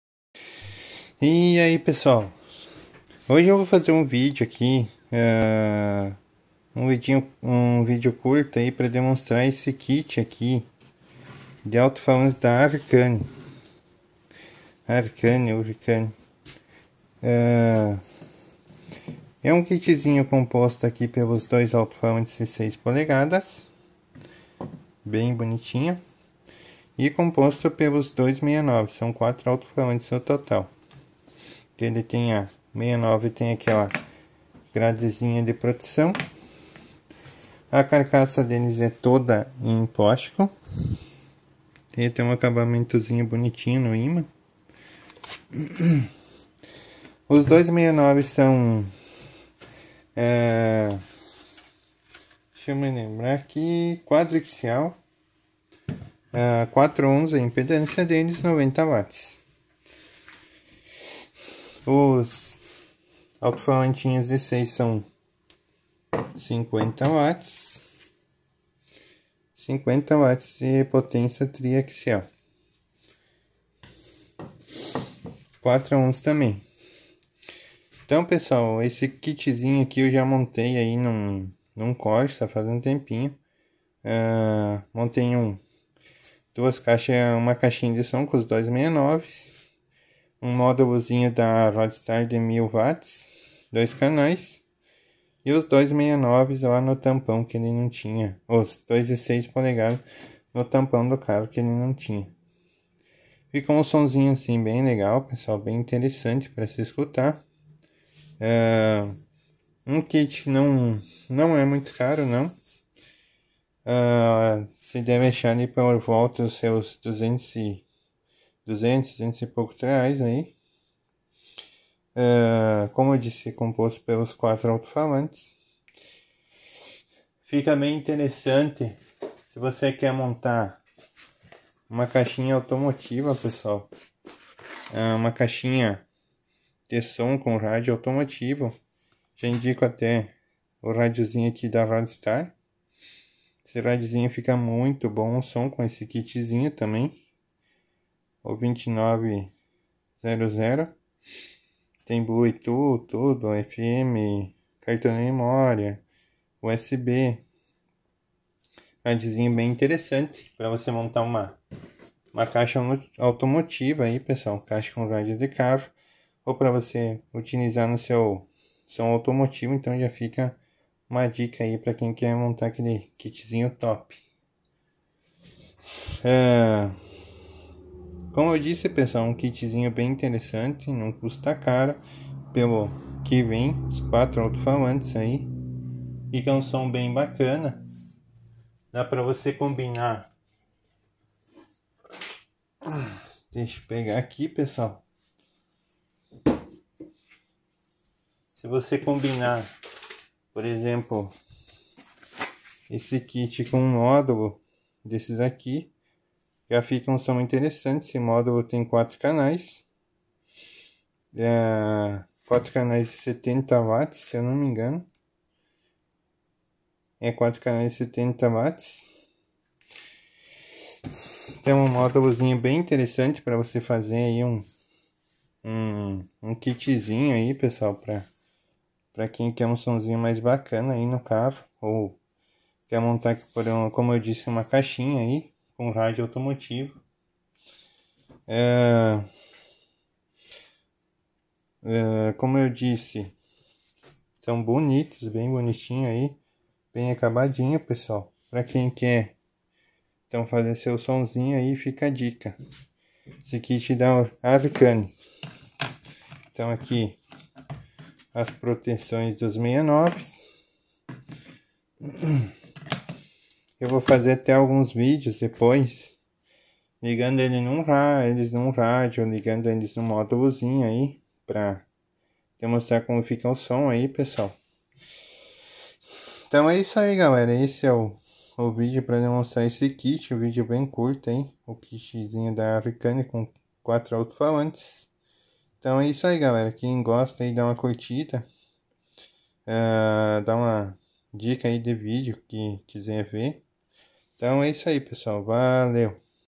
Demonstrando kit De Alto Falantes Hurricane e Dicas Para Montar Seu Som.
Nesse vídeo mostro m kit Hurricane com 2 alto falantes 6 x 9 e 2 alto falantes 6 polegadas , com o total de 280 watts.